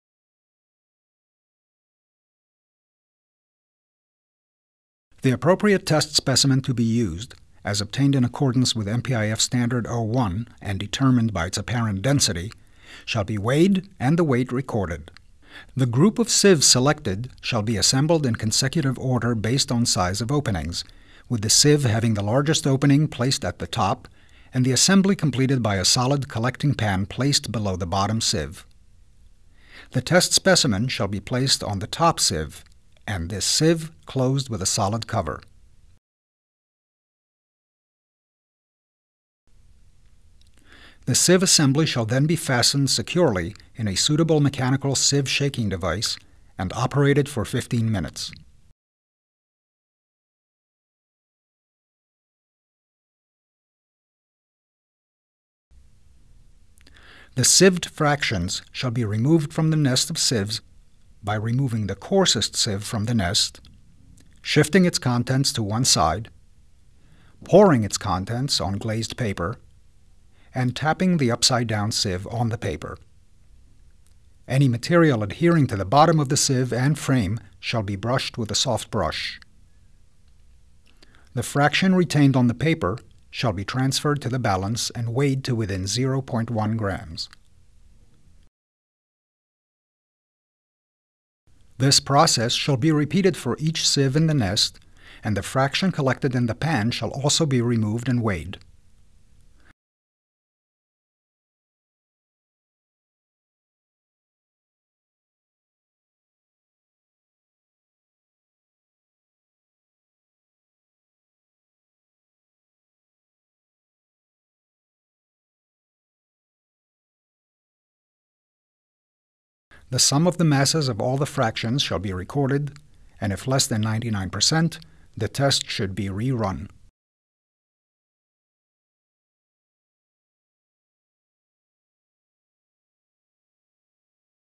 MPIF Standard Test Method 05
From the Standard Test Methods for Metal Powders and Powder Metallurgy Products published by Metal Powder Industries Federation. This is a demonstration of Standard 05: Determination of Sieve Analysis of Metal Powders. For more information on MPIF Standard Test Methods, visit mpif.org